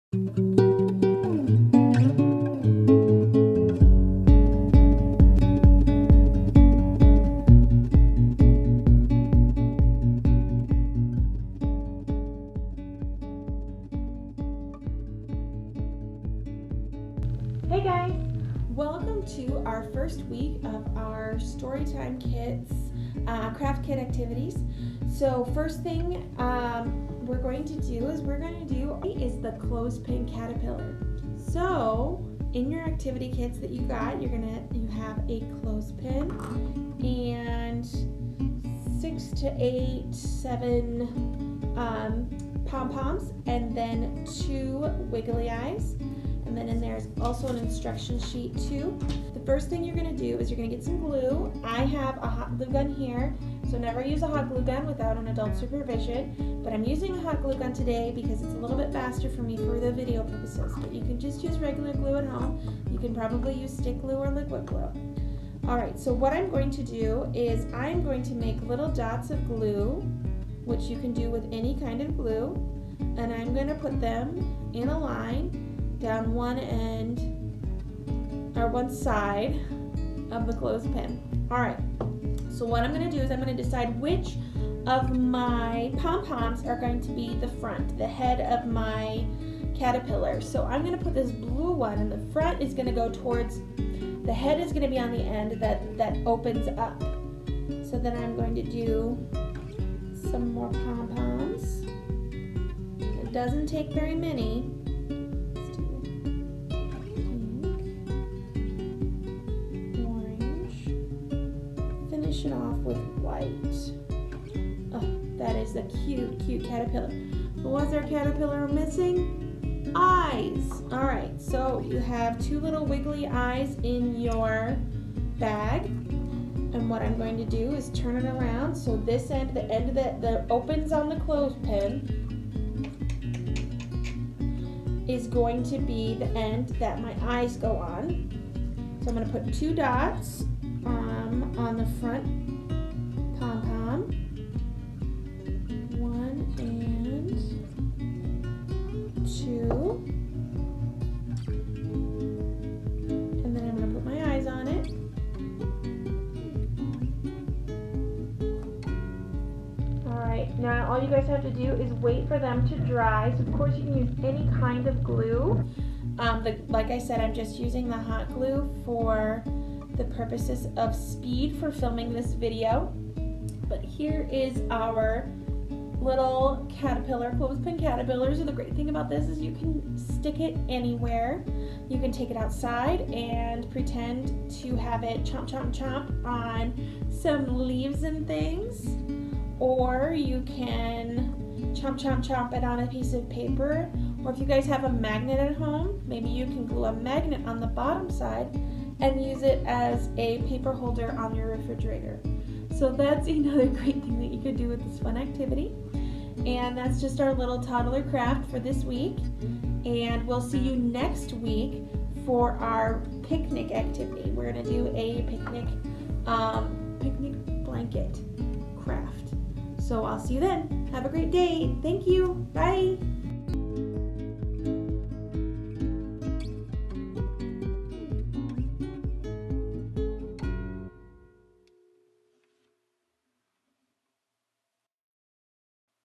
Clothespin Caterpillar Craft
Sumner Public Library StoryTime: Week 1 craft kits
• Clothespin
• Colored pom poms
• Wiggle eyes- 2

1. Choose the order you want your caterpillars pom poms to be in.
2. Glue pom poms on to clothespin.
3. Glue wiggle eyes on end pom pom closest to the opening end of the pin.